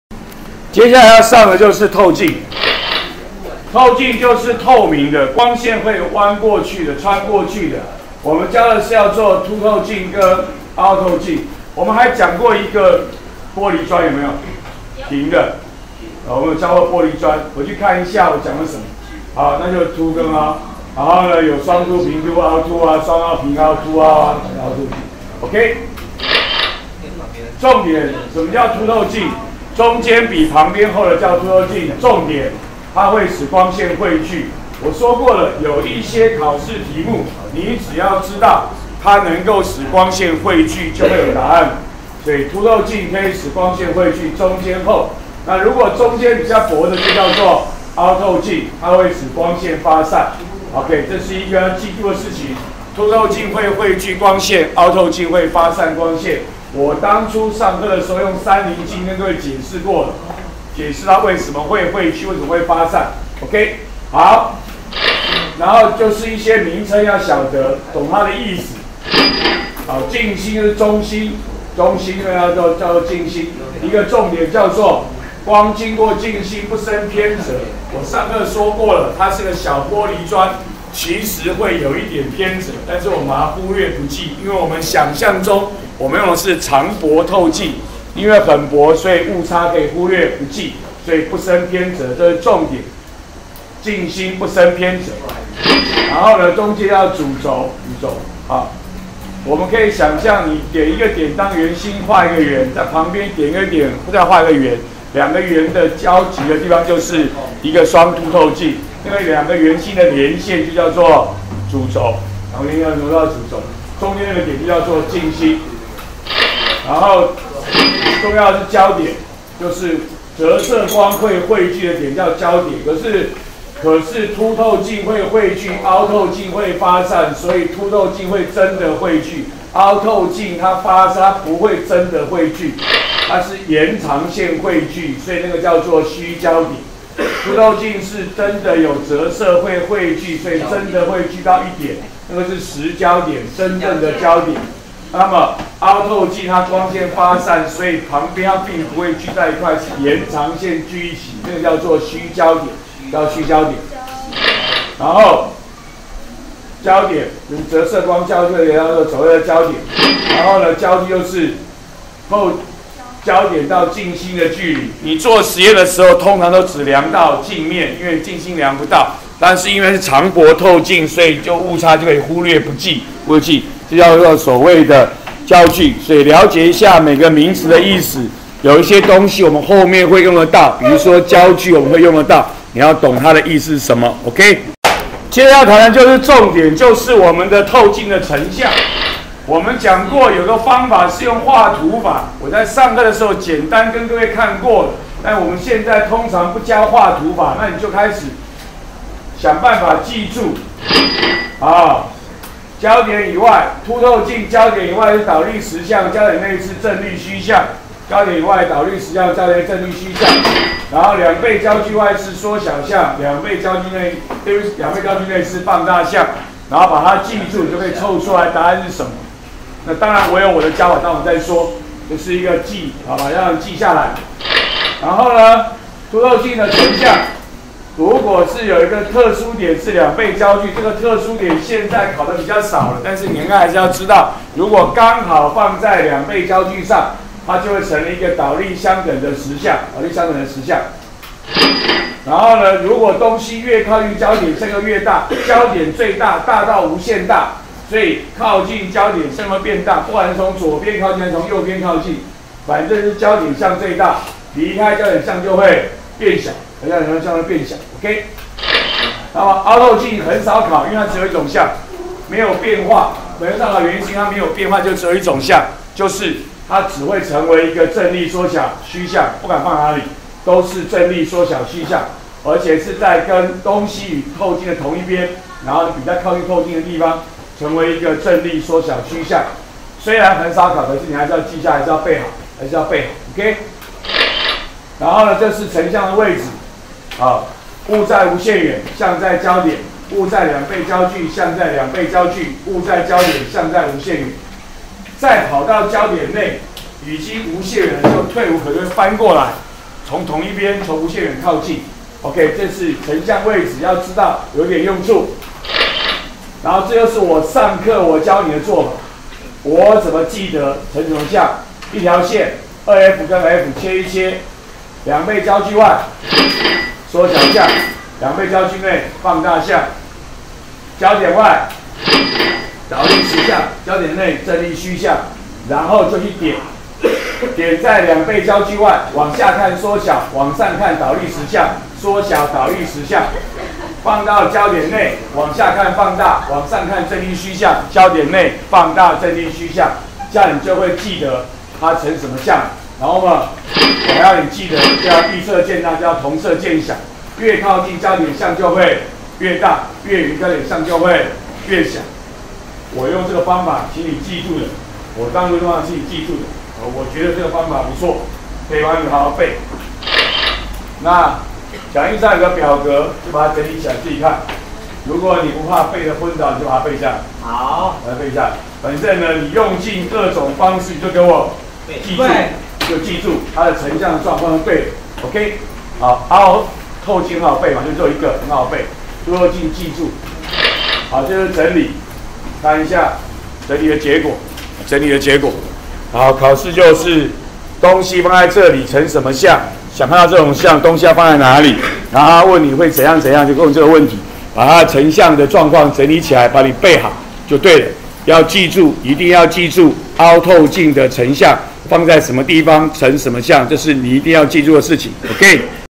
103學年複習光16：透鏡意義、種類、部位名稱、成像
說明何謂透鏡，介紹凹凸透鏡，說明透鏡各部位的名稱及意義。介紹凹凸透鏡的成像，提示記憶方法。